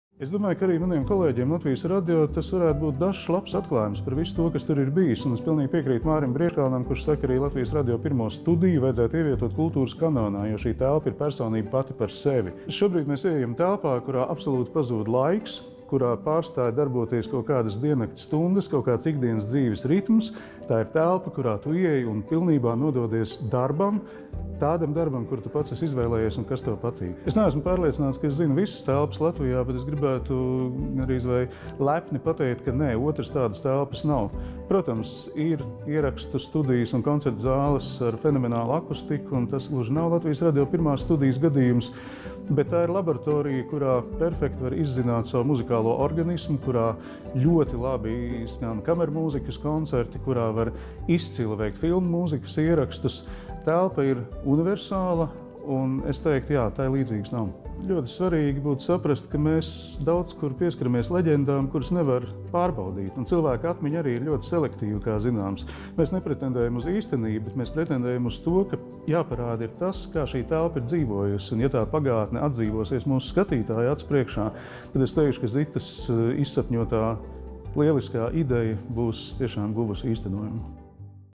Orests Silabriedis: Latvijas Radio 1. studija ir personība pati par sevi
Latvijas Radio 3 "Klasika" programmu vadītājs un filmas "Pirmā studija" redaktors Orests Silabriedis atzīst: "LR 1. studija ir telpa, kurā absolūti pazūd laiks, pārstāj darboties diennakts stundas, ikdienas dzīves ritms. Tā ir telpa, kurā tu ieej un pilnībā nododies darbam. Tādam darbam, kuru esi izvēlējies un kas tev patīk."